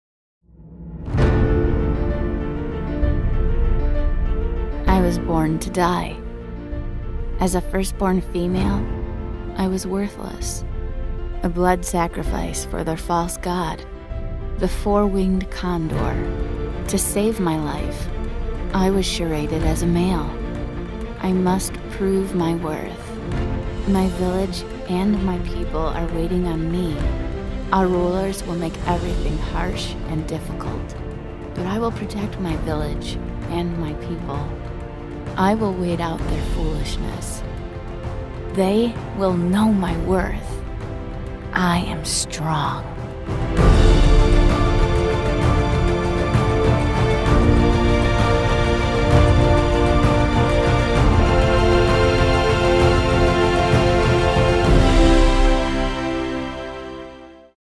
Firstborn by Lorie Ann Grover (Official Trailer)
In Lorie Ann Grover's new YA novel, Tiadone has been forced to live her entire life as a female accepted as male in her community in order to survive as a firstborn child. But when she needs to pass the rites of manhood, she finds the Creator may have use for her feminine traits after all.